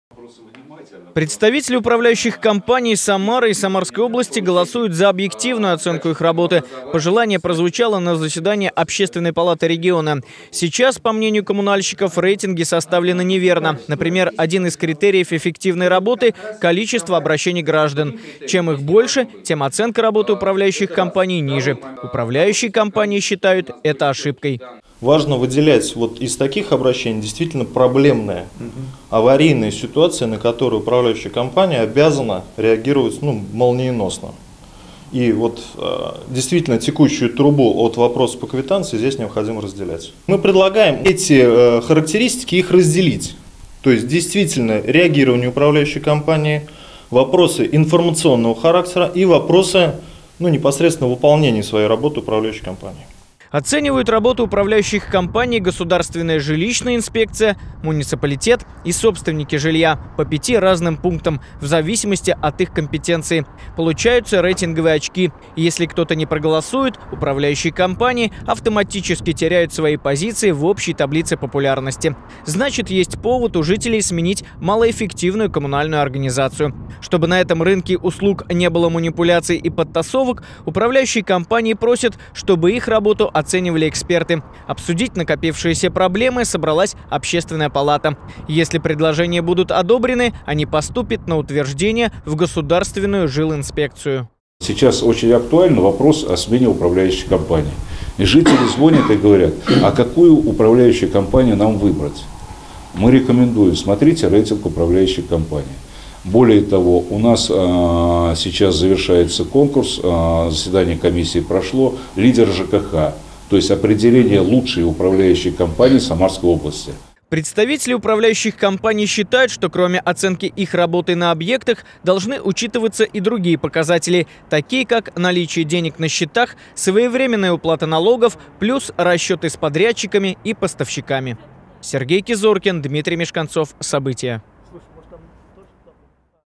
Хороший рейтинг нужно заслужить. В Самаре по-новому будут оценивать работу управляющих компаний
Поставили оценку. От рейтинга управляющих компаний зависит, будут ли они работать на рынке предоставления коммунальных услуг в регионе или нет. Кто оценивает труд коммунальщиков и по какой шкале — расскажет Сергей Кизоркин.